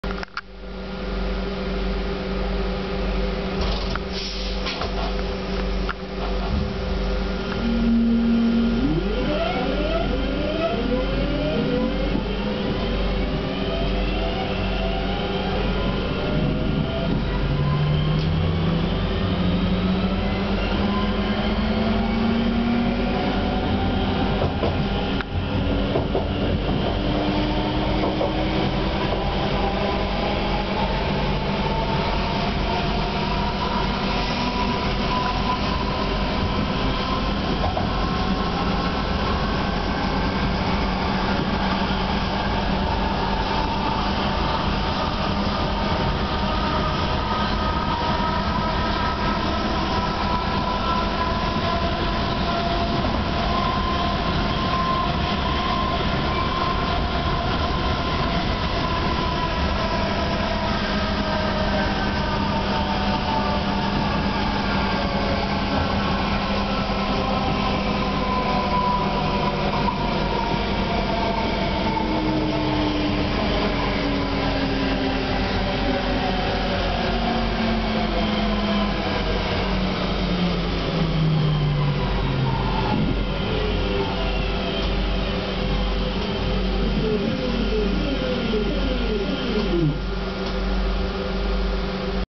Onboard London Midland Class 323 - Aston to Witton
Onboard a London Midland Class 323 - Aston to Witton on 10/05/08.